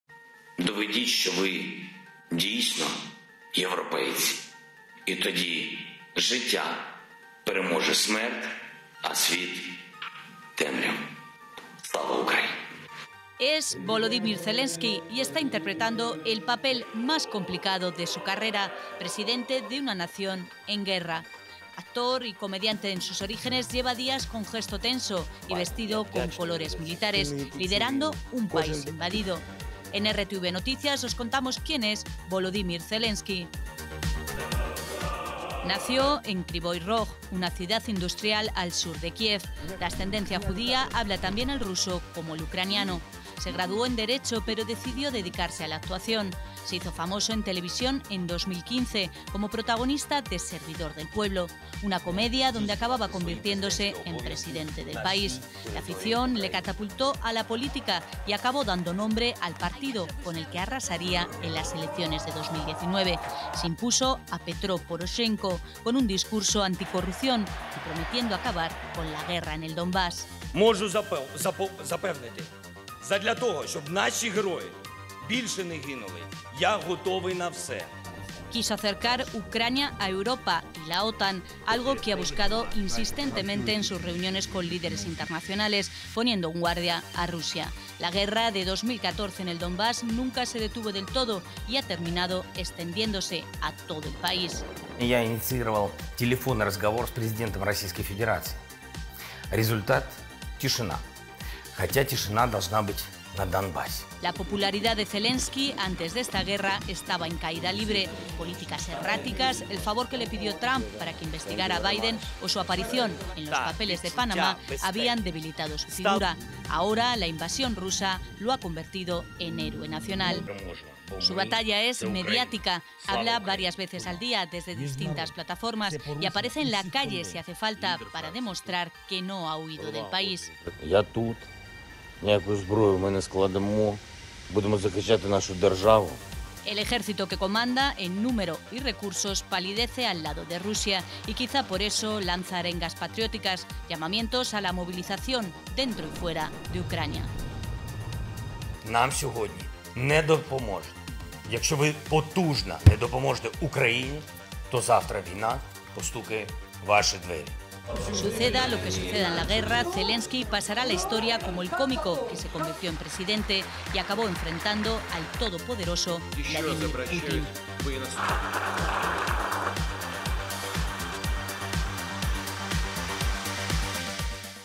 VOLODÍMIR ZELENSKI: De actor a presidente de Ucrania enfrentado a Putin | RTVE Noticias
El presidente de Ucrania, Volodímir Zelenski, está interpretando el papel más complicado de su carrera. En el pasado fue cómico y actor, pero su figura nunca había sido tan mediática como en este momento. En medio de un conflicto retransmitido en vivo, ha pasado de ser un presidente cuestionado para convertirse en el líder de un país invadido.

Zelenski llegó al poder en 2019, tras barrer en las elecciones a su rival, Petró Poroshenko. Su rostro era familiar para los ucranianos tras protagonizar “Servidor del Pueblo”, la comedia televisiva que predijo su ascenso a la política. En ella interpretaba a un sencillo maestro que llegaba a convertirse, de forma inesperada, en presidente de Ucrania.

Con su discurso contra la corrupción y su intención de acabar con la guerra en el Donbás conquistó a una población descontenta con la élite gobernante. Defensor de acercar Ucrania a Europa y la OTAN, ha desarrollado una intensa actividad diplomática para conseguirlo. A pesar de todo, antes del estallido de esta guerra su popularidad estaba en caída libre por una serie de políticas erráticas fruto de su inexperiencia.

Ahora, el conflicto lo ha hecho resurgir como símbolo de la nación. Independientemente de lo que suceda, pasará a la historia como el cómico que se enfrentó a la todopoderosa Rusia de Putin.